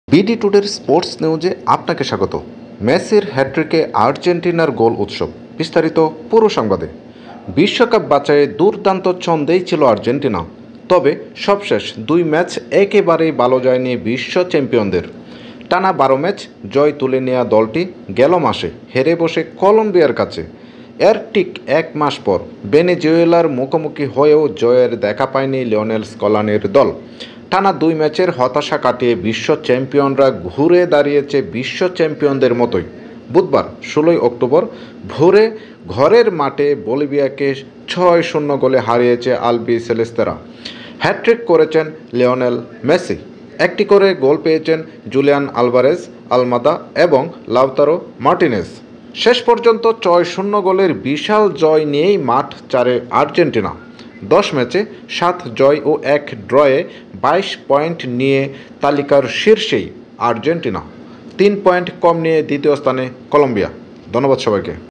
Sports news today | Messi hat-trick | Argentina vs Bolivia
Bd Today is a sports News Channel.Sports news today | Messi hat-trick | Argentina vs Bolivia

Messi's hat-trick is Argentina's goal celebration

 Argentina was in great shape in the World Cup qualifiers.  However, the last two matches did not go well for the world champions.  The team, which won 12 matches in a row, lost to Colombia last month.  Just over a month later, Lionel Scaloni's side went winless against Venezuela.

 The world champions have come back as world champions after a two-game losing streak.  Albiceleste defeated Bolivia 6-0 at home on Wednesday, October 16.  Lionel Messi scored a hat trick.  Julian Alvarez, Almada and Lautaro Martinez scored a goal each.

 In the end, Argentina left the field with a huge victory of 6-0 goals.  Argentina tops the points list with 22 points from 7 wins and 1 draw in 10 matches.  Colombia is in second place with 3 points less.